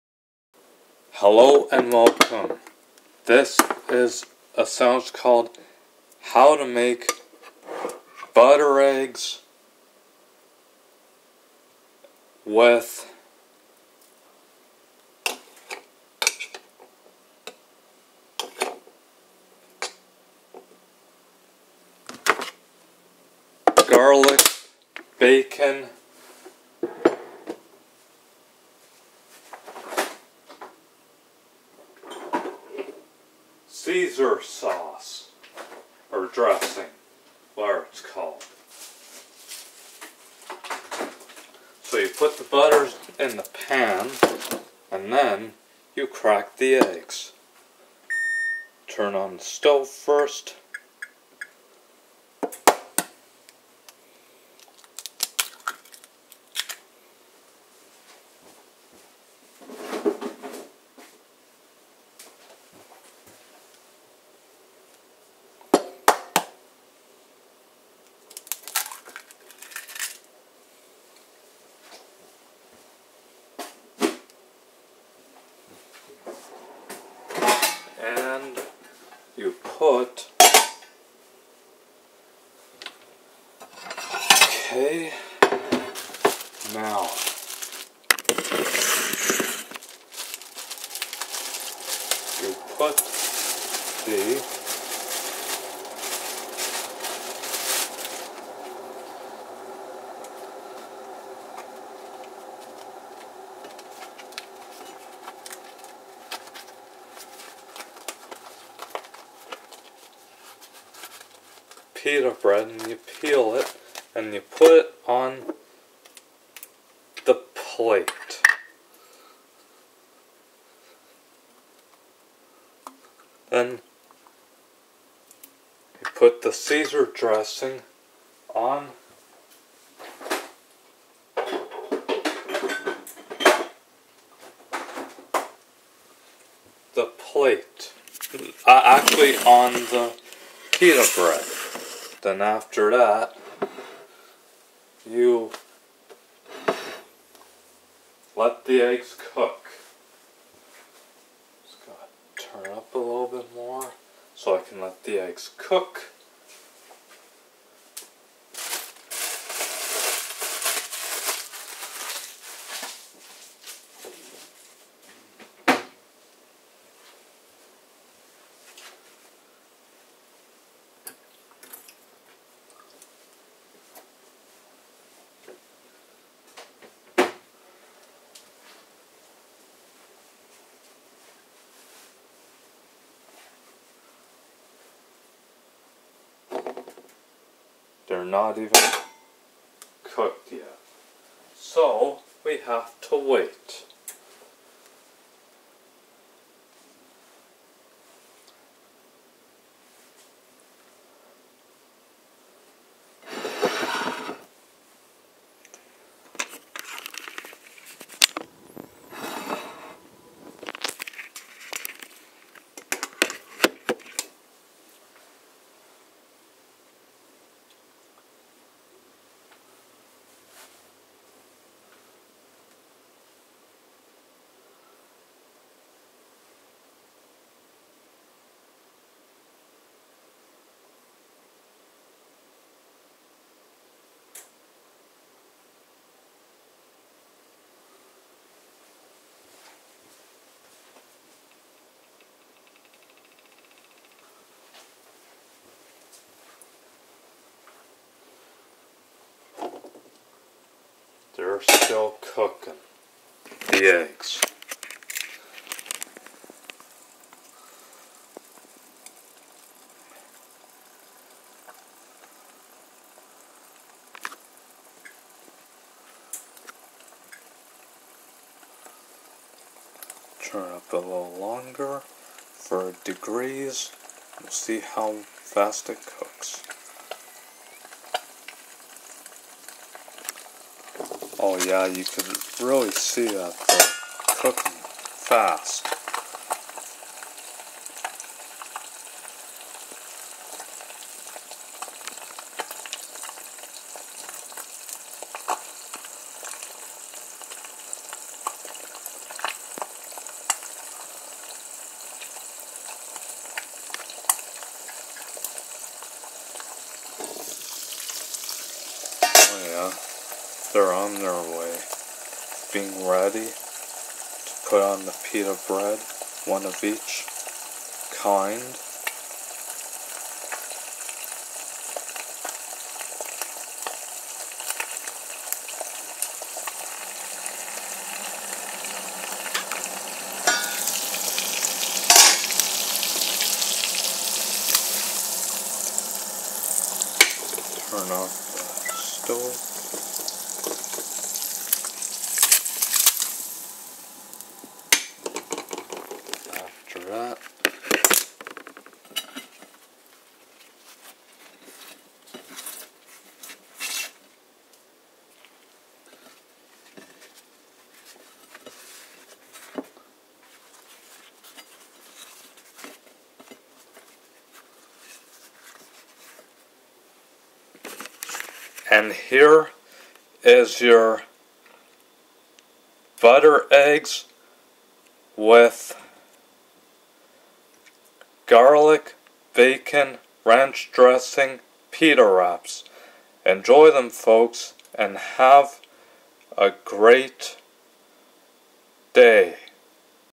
How to Make Butter Eggs & Garlic Bacon Caesar Dressing Wraps
1. Get the pita bread & peel it into 2 pitas.
2. Get the pans & Put butter in the pans.
3. Crack 2 eggs & put it in each pan & let it cool for a couple of moments.
4. Put Garlic Bacon Caesar Dressing onto the pita bread for each one.
5. Turn off the stove & one of each egg on the pita bread.
6. Wrap up the pita bread for both & Here's the Butter Eggs & Garlic Bacon Caesar Dressing Wraps, Enjoy them folks.